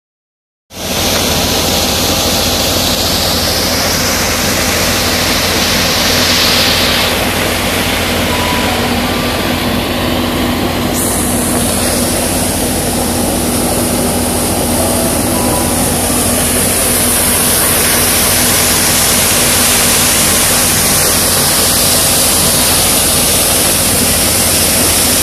install dan test Excavator pump dredger cutter section pompa sedot pasir lumpur di Pelabuhan Manado
JUAL POMPA SEDOT PASIR LUMPUR , bisa digabungkan dengan excavator, tidak perlu powerpack lagi. Dilengkapi dengan cutter section, sehingga dengan goyangan arm excavator akan menambah hasil lebih maksimal.